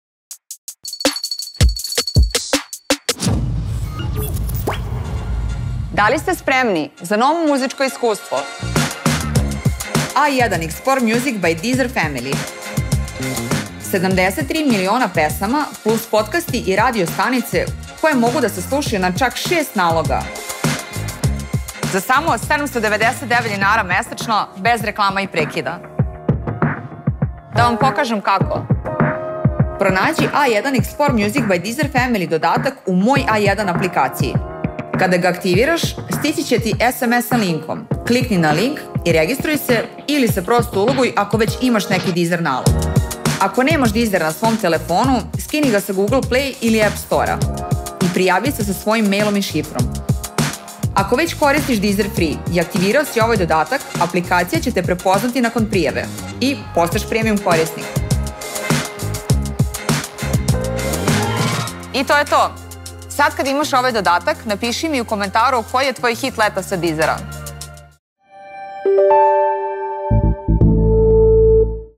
A1 Xplore Music by Deezer Family
Najpopularnija Deezer aplikacija za strimig muzike je sada dostupna A1 postpaid korisnicima. Vreme je da pronađeš nove omiljene pesme, kao i neke stare favorite u moru od 73 miliona muzičkih numera i velikom izboru podkasta. Probaj, prvih 30 dana je bez naplate!